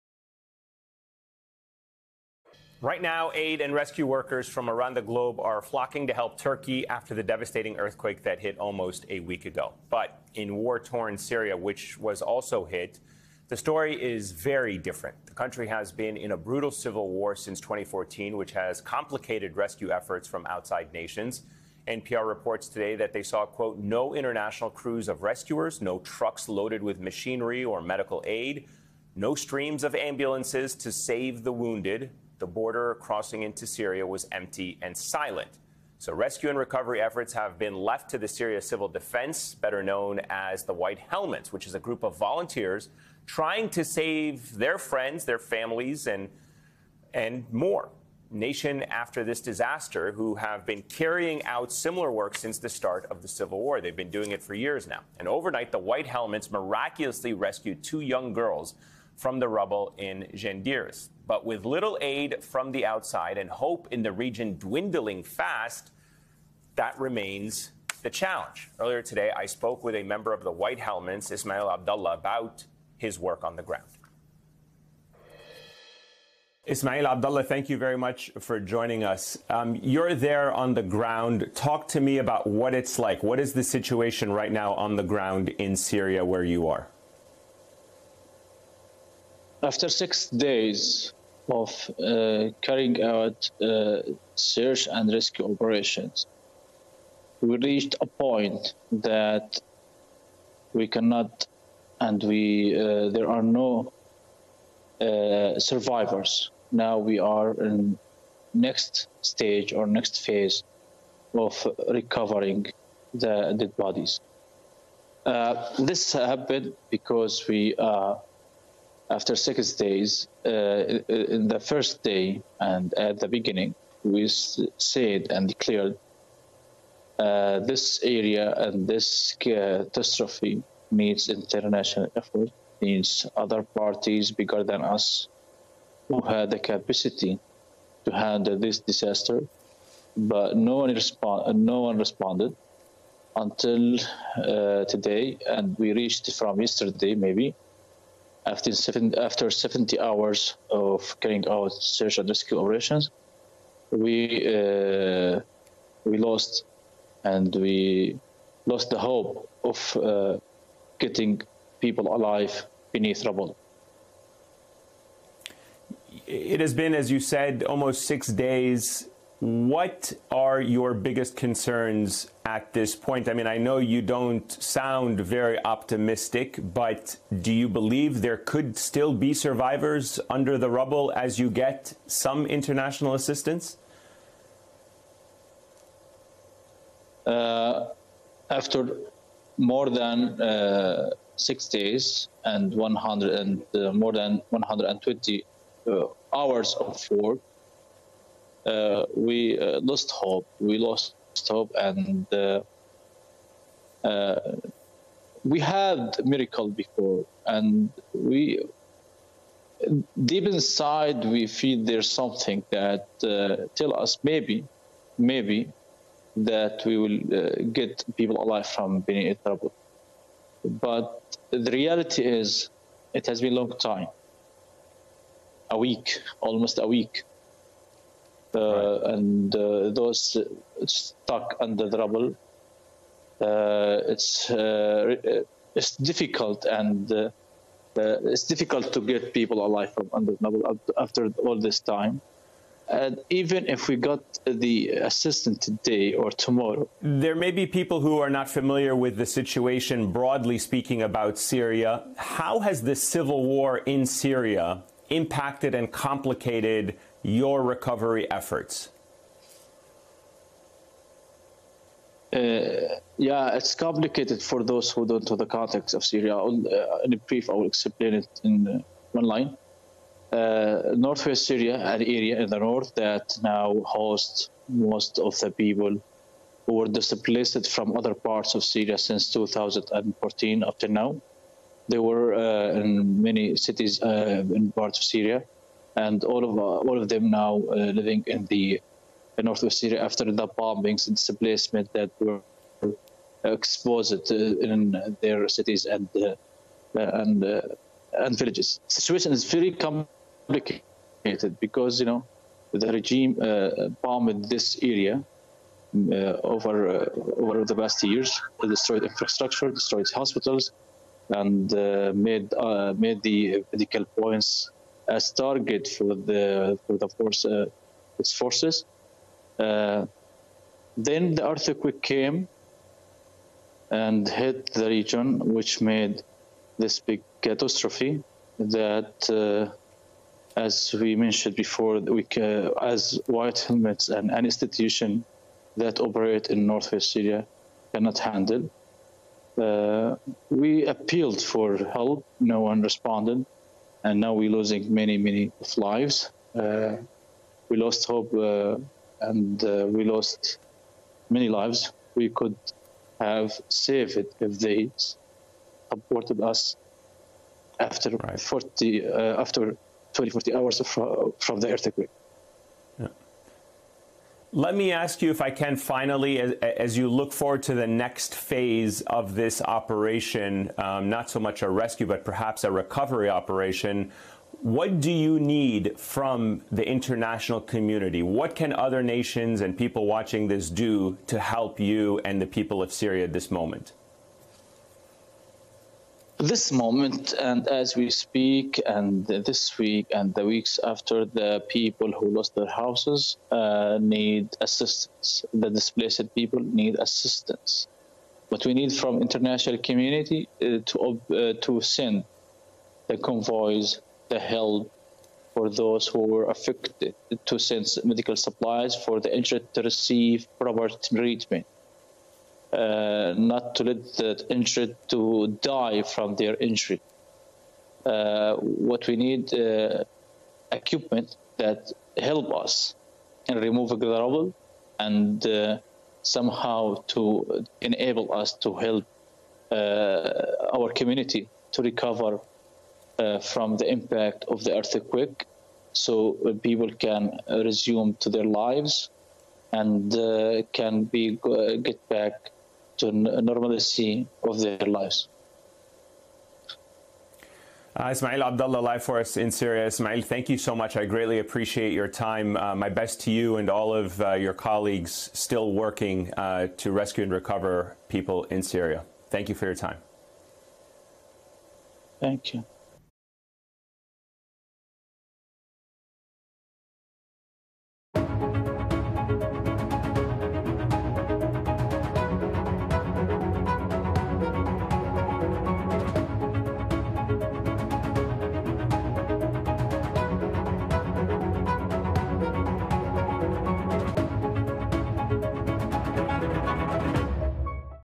Syrians plead for help after earthquakes cause widespread devastation
The major earthquakes in Turkey and Syria have caused widespread devastation. Nations all around the globe are helping Turkey recover but the civil war in Syria is making outside help nearly impossible. MSNBC’s Ayman Mohyeldin spoke with White Helmet volunteer, Ismail Alabdullah, who has been trying to pull survivors from the rubble, about what it’s like there on the ground in Syria.